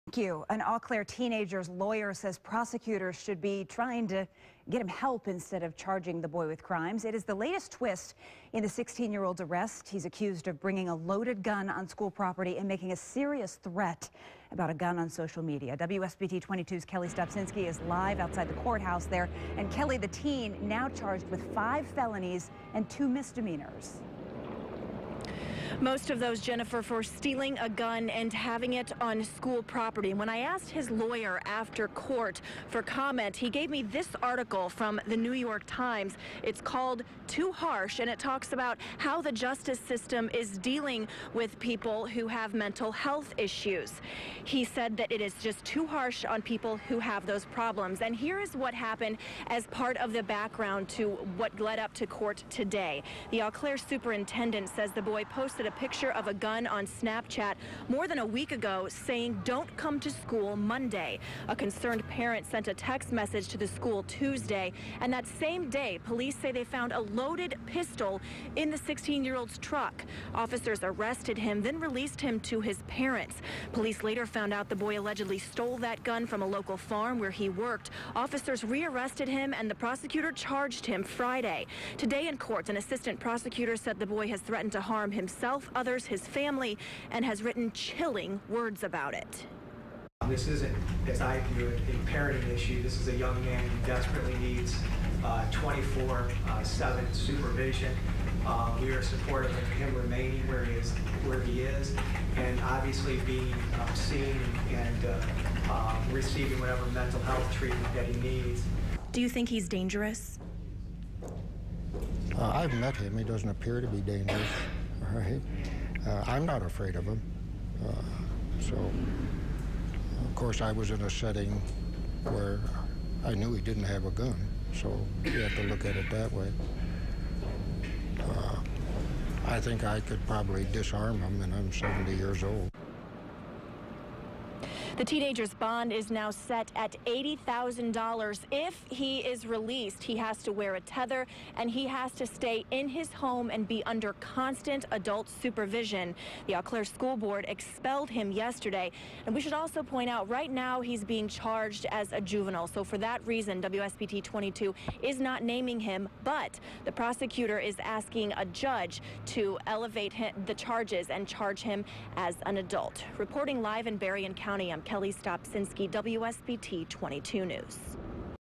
Eau Claire teen appears in court on gun charges
An Eau Claire teenager's lawyer says prosecutors should be trying to get him help instead of charging the boy with crimes. _________________